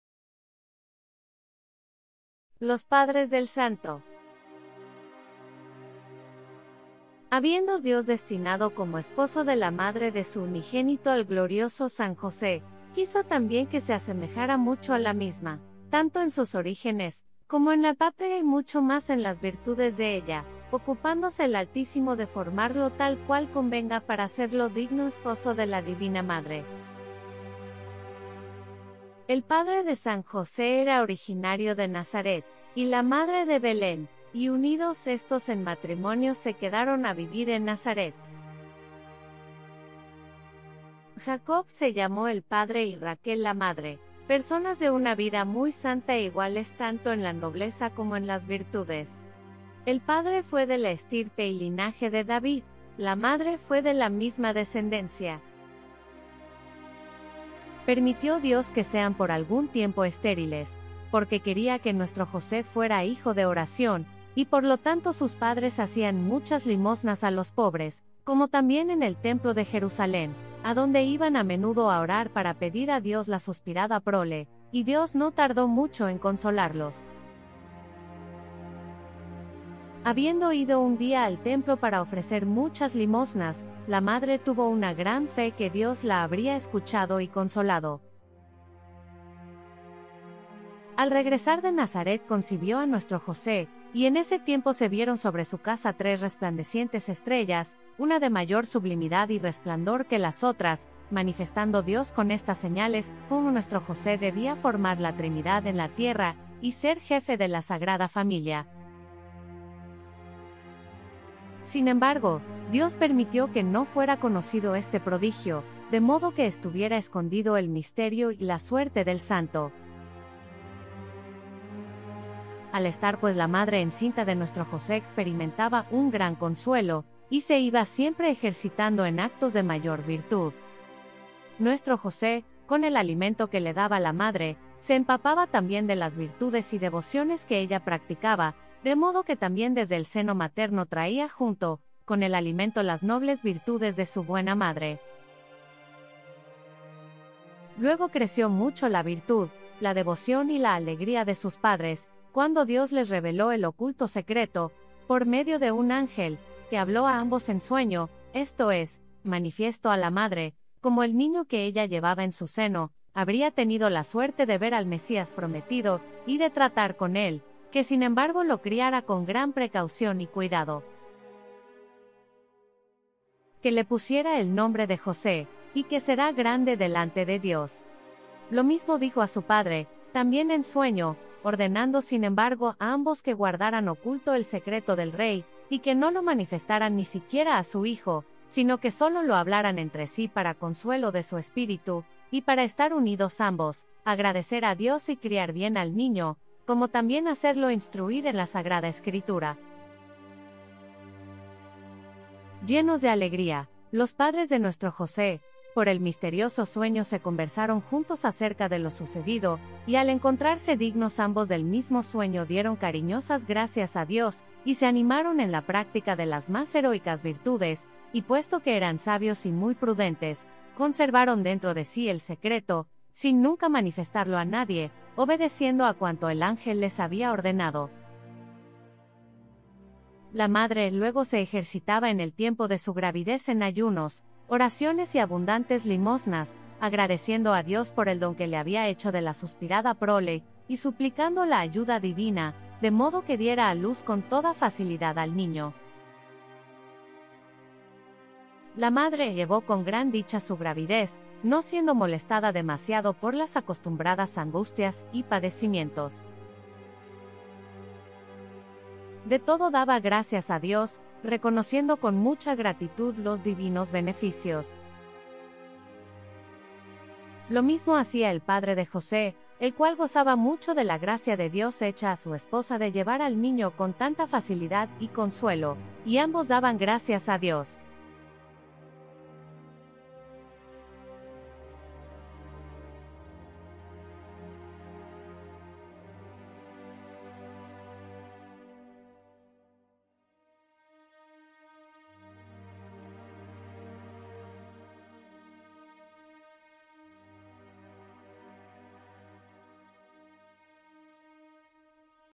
Vida de San José revelada a la Hna. Mª Cecilia Baij - TOMO I CAPITULO I- Los Padres del Santo
Vida de San José - "Desde el Nacimiento de José hasta la Encarnación"- revelada a la Hermana María Cecilia Baij, Sierva de Dios  - (vivió entre el 1694 y el 1766) en el Monasterio Benedictino de MONTEFIASCONE, Italia.